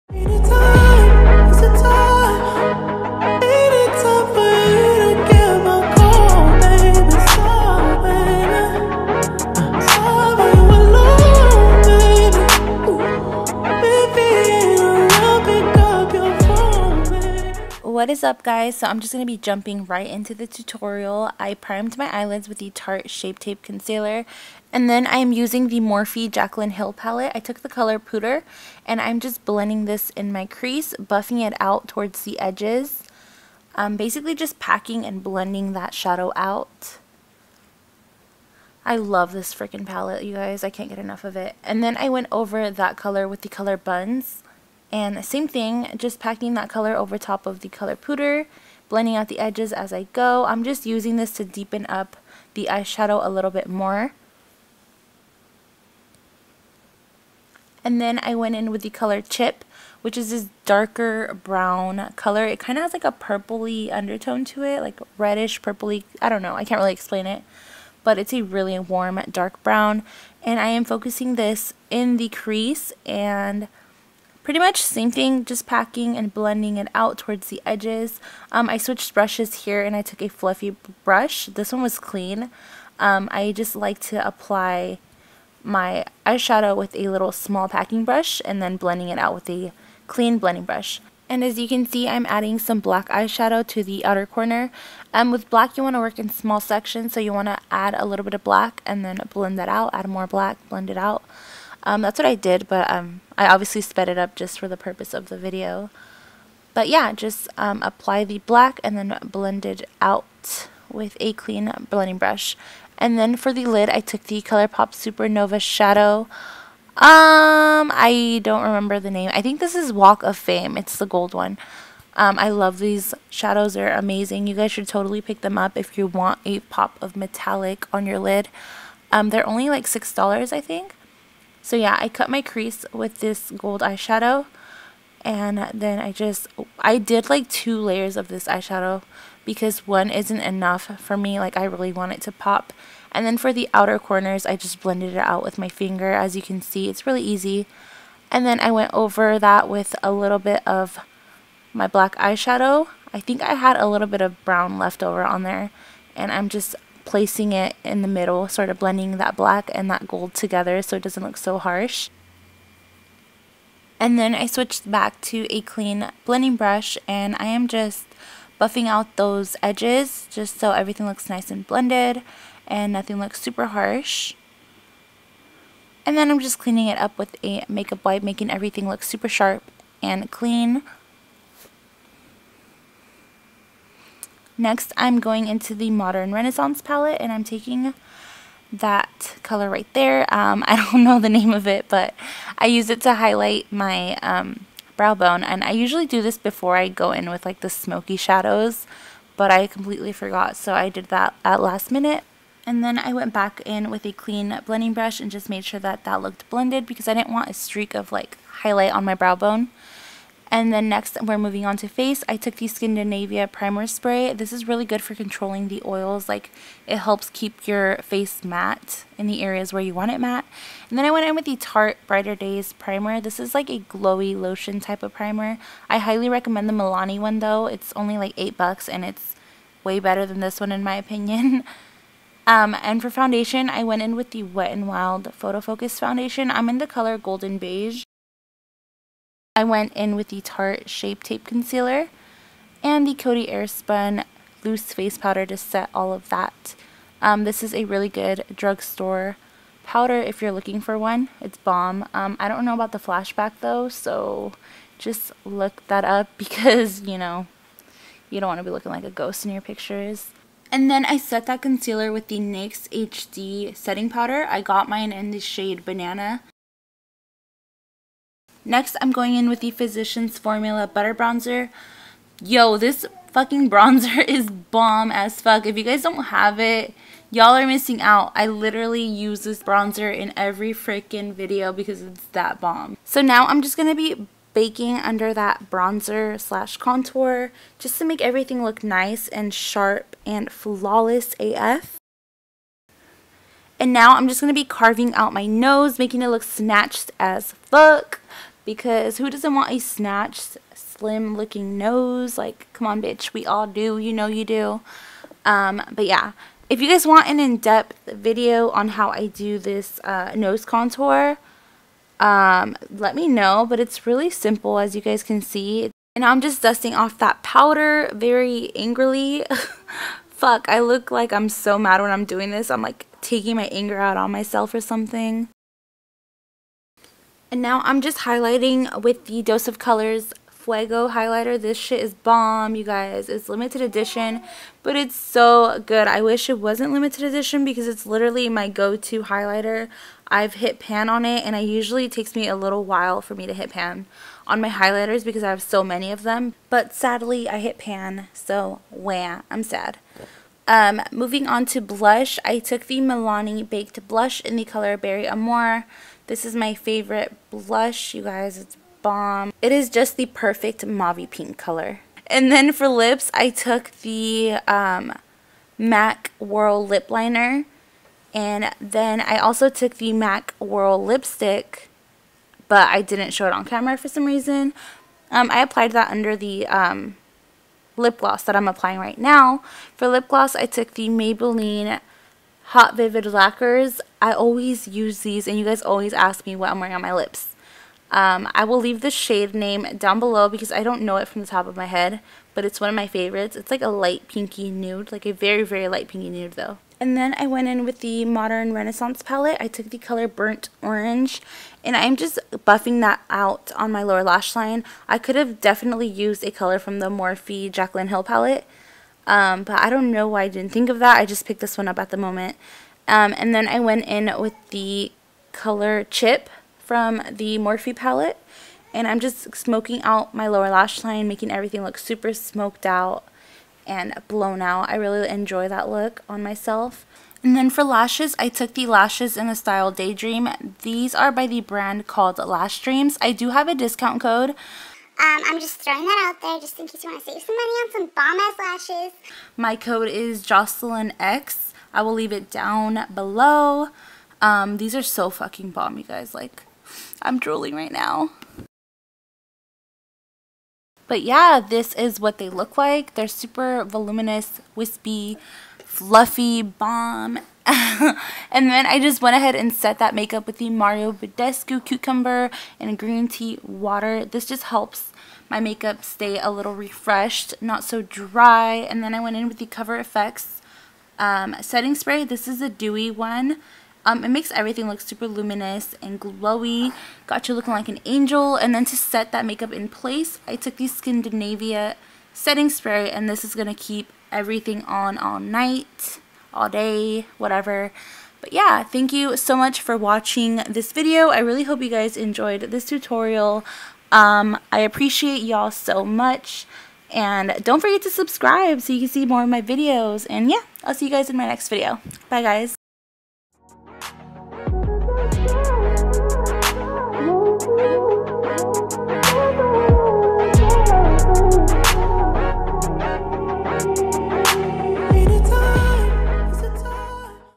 GLAM AF GOLD HALF CUT CREASE | Jocelyn Sanchez
so one of my eyes was being stubborn af & for some reason the eyeshadow wasn't wanting to blend out buuuut just ignore that ok. lol

VV LASHES USED IN TUTORIAL VV

Products used-

LIPS:
Maybelline Vivid hot lacquer "Tease"